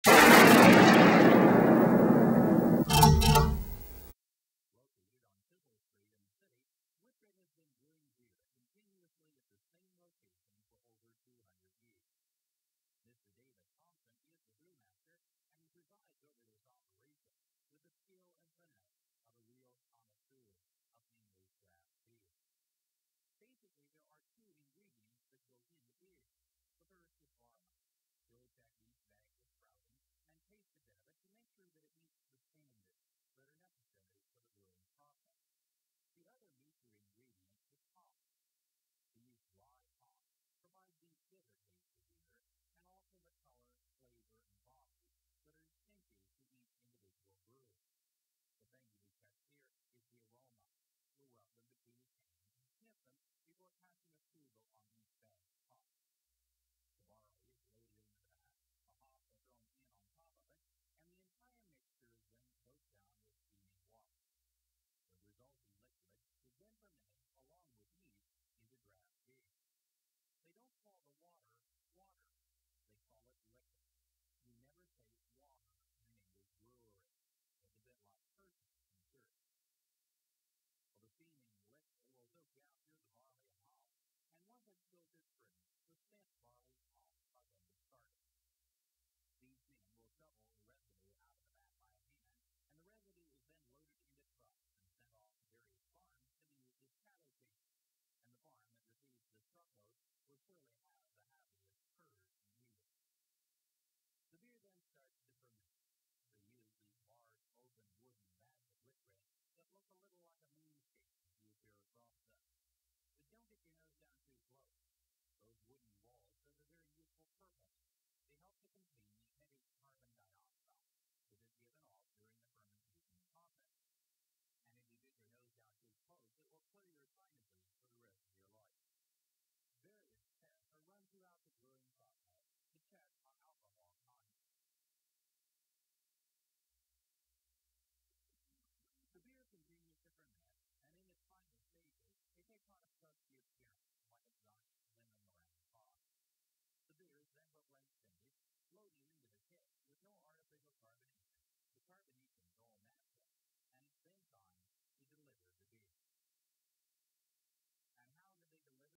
Brewery Interior 1 - Making Beer - Kettles - Factory - Best Shot Footage - Stock Footage
Interior of Brewery, Kettles. A brewery is a dedicated building for the making of beer, though beer can be made at home, and has been for much of beer's history. A company which makes beer is called either a brewery or a brewing company. The diversity of size in breweries is matched by the diversity of processes, degrees of automation, and kinds of beer produced in breweries. Typically a brewery is divided into distinct sections, with each section reserved for one part of the brewing process.